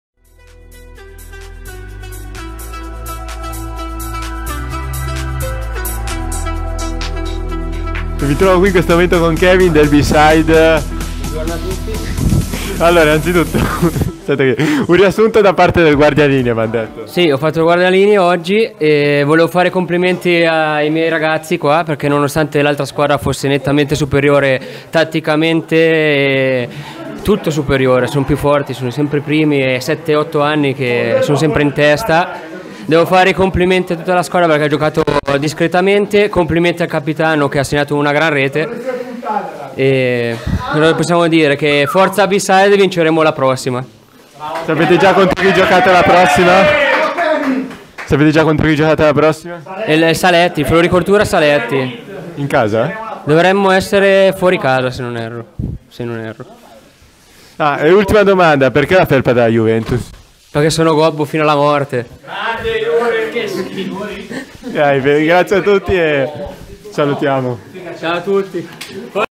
spogliatoio CSI B SIDE
Nonostante la sonora sconfitta di 8- 1 contro il PESCE D'ORO, la squadra non si arrende e ci rilascia un'intervista stile B-SIDE… Buona visione